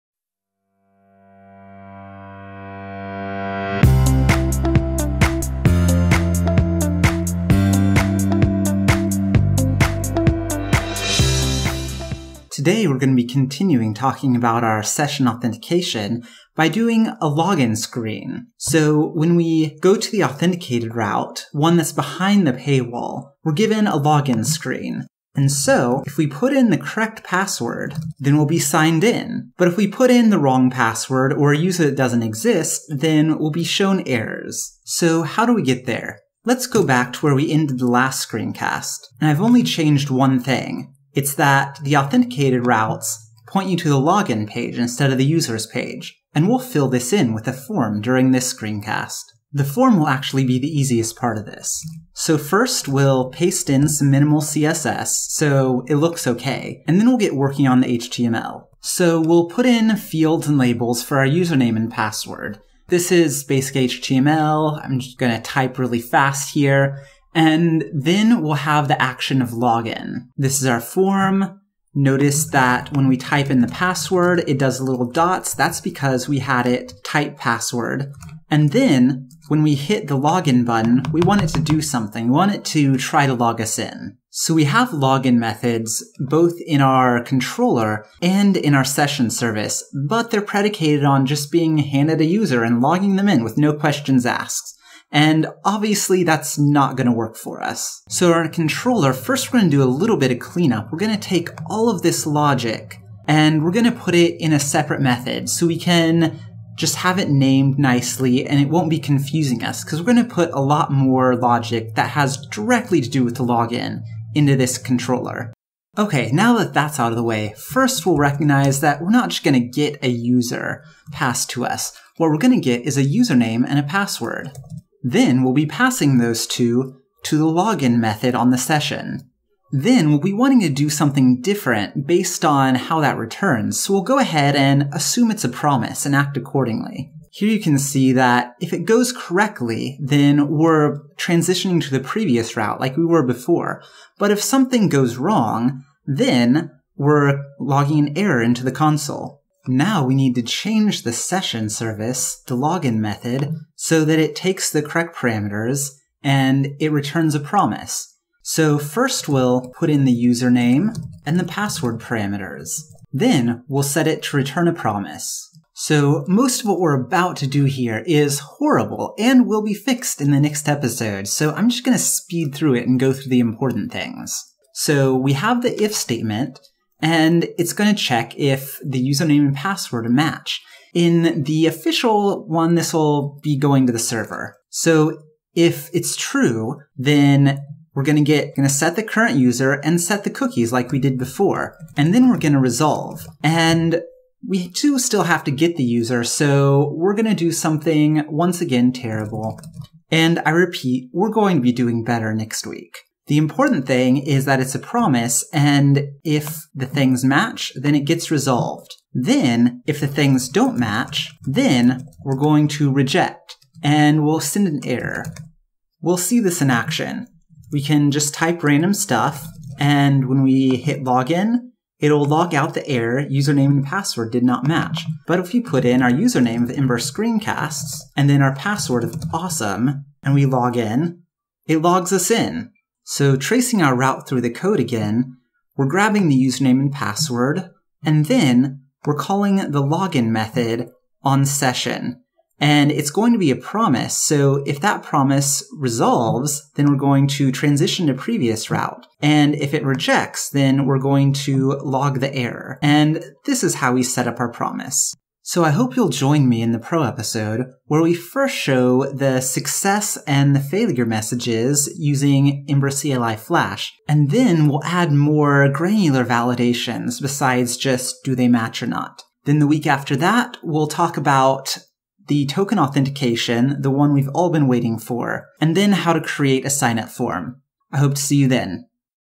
EmberScreencast 86 - Login Form
The login form is the gateway to your application. Make sure it works right.

In this episode we create the template for a login form, and give it some toy functionality that we will expand upon in upcoming episodes.

Want to learn Ember 2?